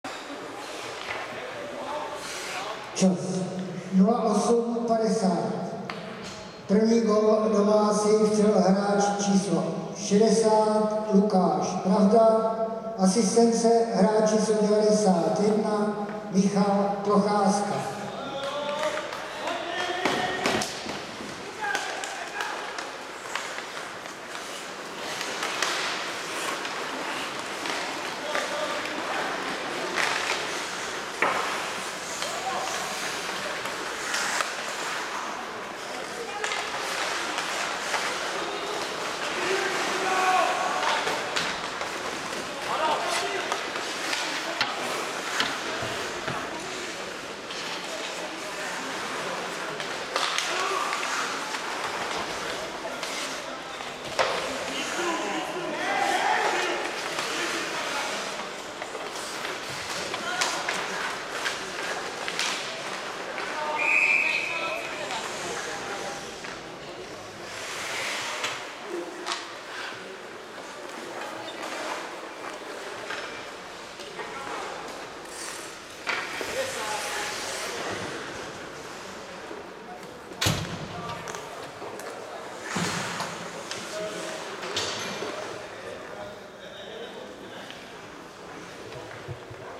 20101105 HK SU_0010.MTS
HC Letci Letňany Praha vs. HC VCES Hradec Králové Extraliga Starší Dorost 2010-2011 Hokej Czech Midget 18U Extra League Ice Hockey Prague Canon Legria HF S21